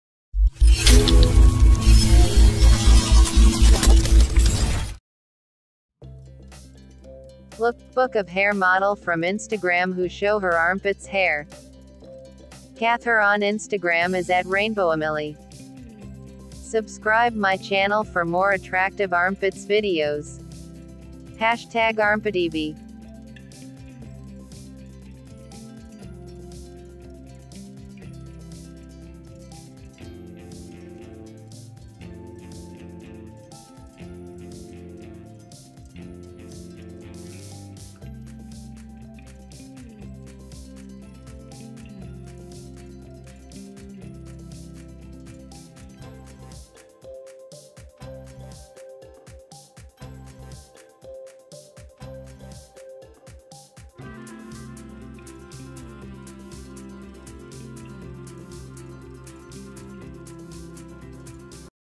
Insta Armpit Hair Model
Lookbook Of Hair Model From Instagram Who Show Her Armpits Hair

Cath Her On Instagram As @rainbowemilly